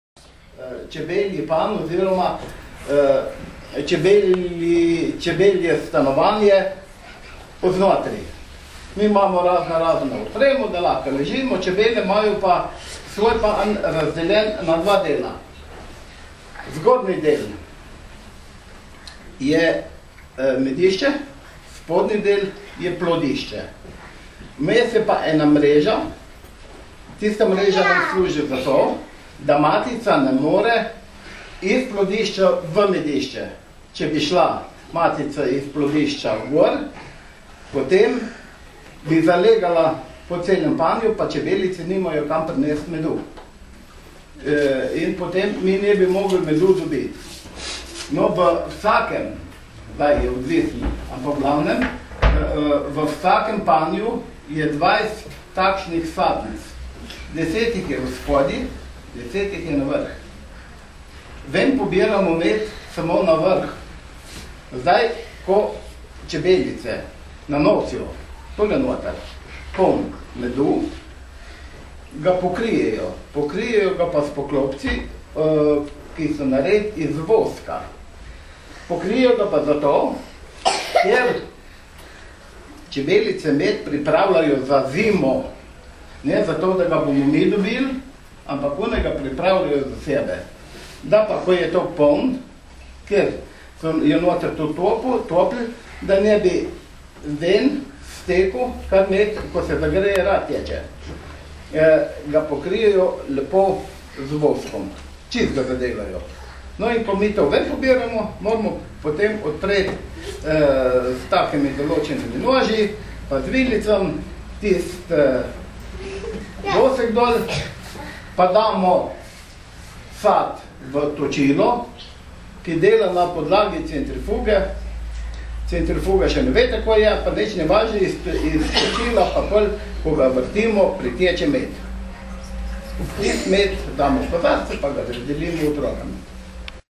Med v vrtcih - razlaga
Predsednik Čebelarskega društva Šentjur Anton Gabršček
@ akcija En dan med v slovenenskih vrtcih